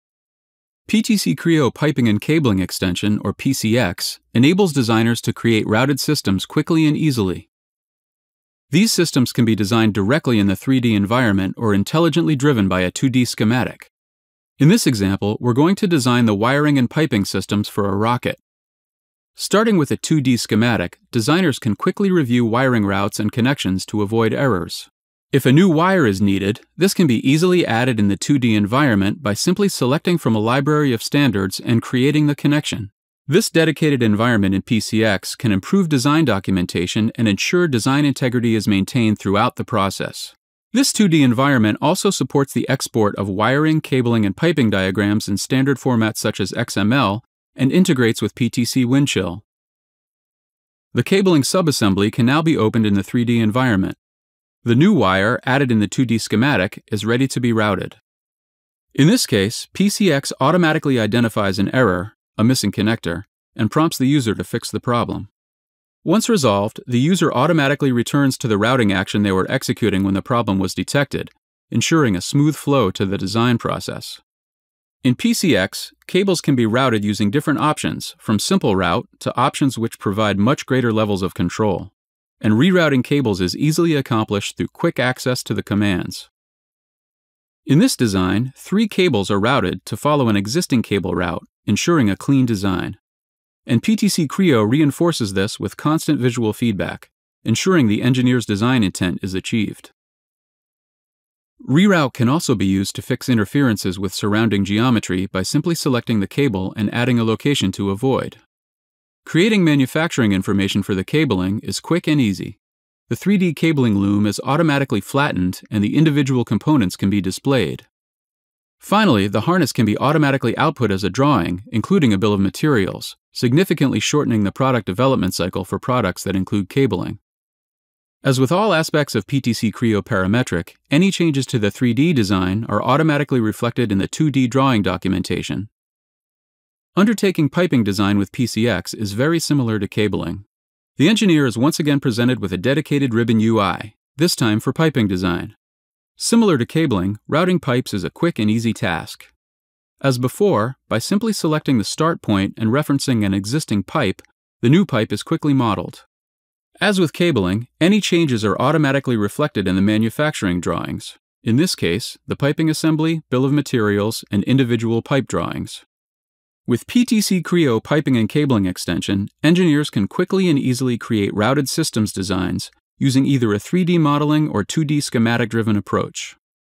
Creo Piping and Cabling Extension
Creo Piping and Cabling Extension (PCX), Automate and Accelerate your 3D Piping and Cabling Design Process.